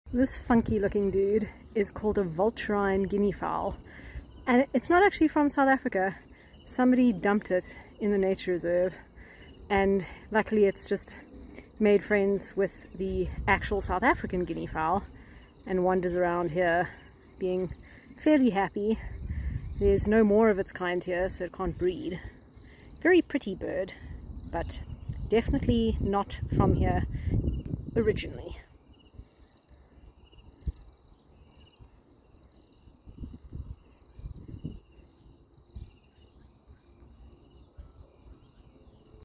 Vulturine Guineafowl dumped in South African Nature Reserve
This Vulturine Guineafowl was dumped in the Faerie Glen Nature Reserve in South Africa.